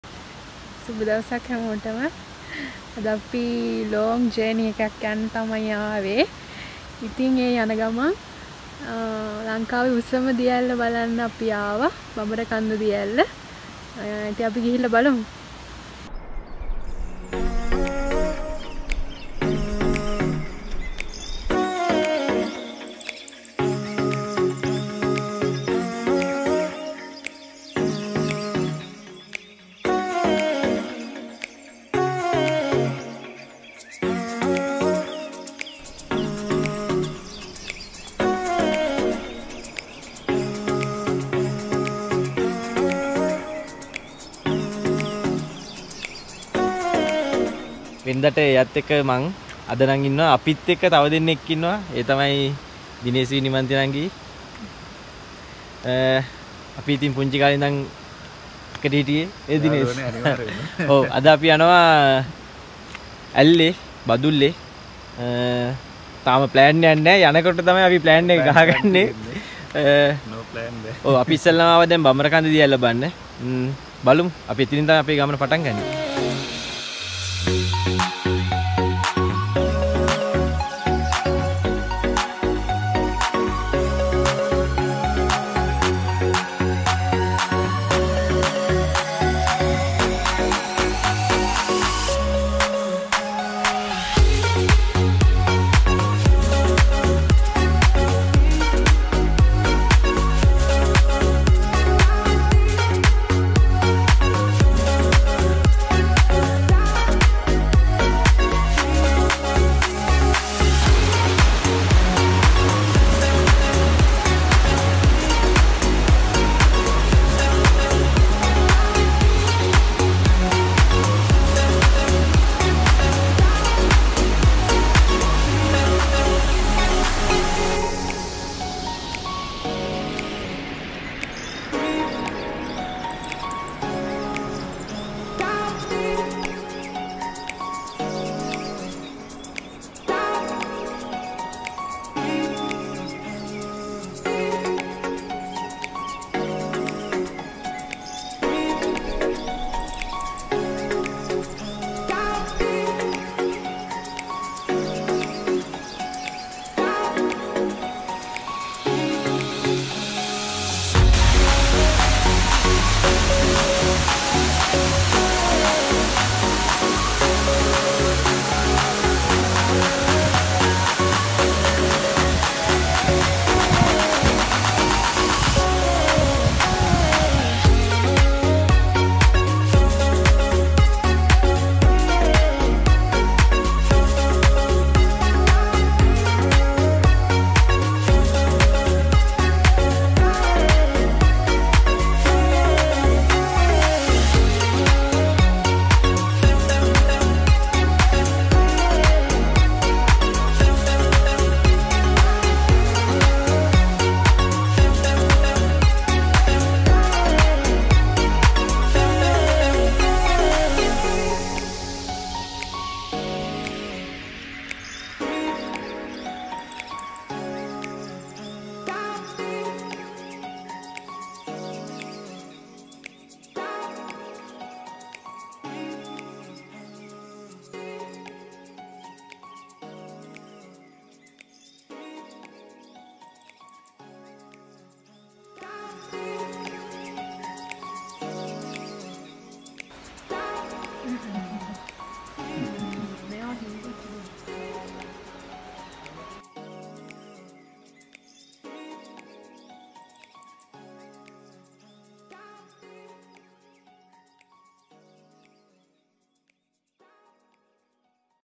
Bambarakanda waterfall | බඹරකන්ද දියඇල්ල | Ella trip (epi 01) | vlog 07
Bambarakanda Falls (also known as Bambarakele Falls) is the tallest waterfall in Sri Lanka. With a height of 263 m (863 ft), it ranks as the 299th highest waterfall in the world[citation needed]. Situated in Kalupahana in the Badulla District, this waterfall is 5 km away from the A4 Highway. The waterfall was formed by Kuda Oya, which is a branch of the Walawe River. The Bambarakanda Falls can be found in a forest of pine trees.